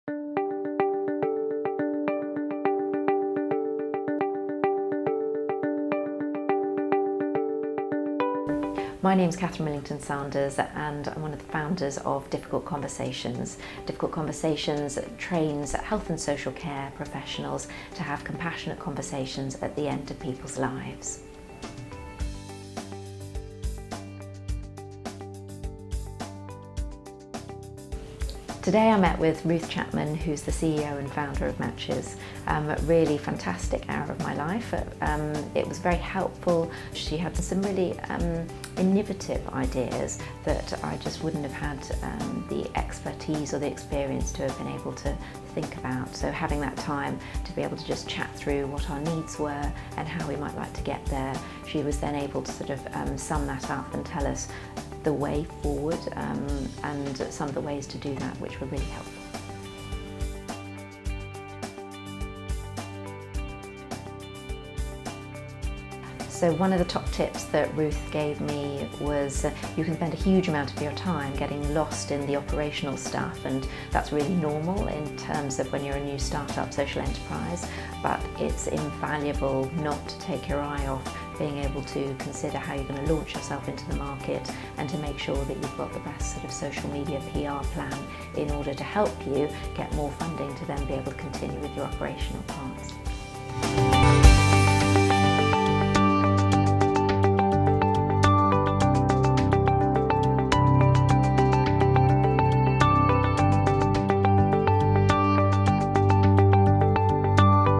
Don't take your eye off your PR plan | Human Lending Library
Catherine Millington Sanders, co-founder of Difficult Conversations, shares her top tip that she took from Ruth Chapman in this video.

As an entrepreneur who is launching a business, it's easy to get caught up in the day to day operational grind of your business. It is really important that you remember to stay focused on your launch and PR strategy as well, if you don't then you risk all your hard work being for nothing!